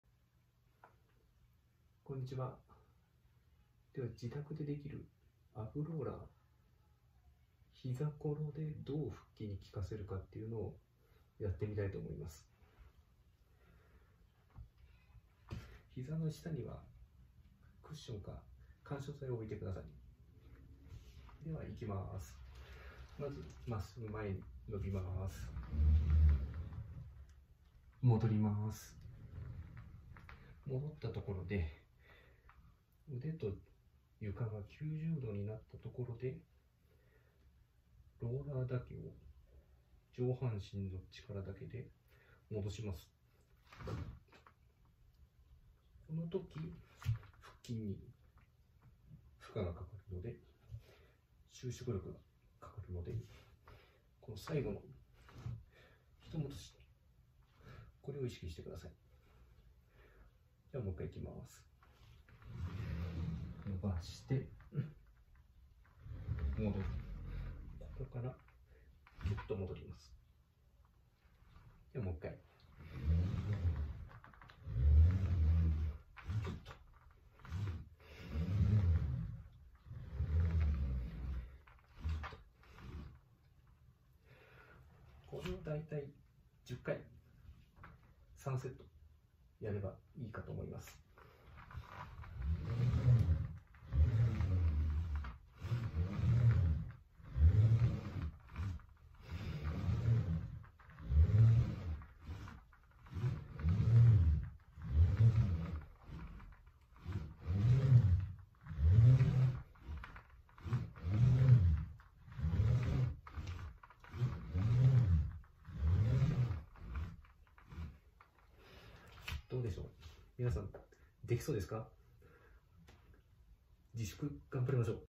アブローラー “膝コロ”
コロナ自粛ですが、自宅でできる自重トレーニングのひとつアブローラーについて。
膝をついた膝コロで効果を上げる方法の説明です。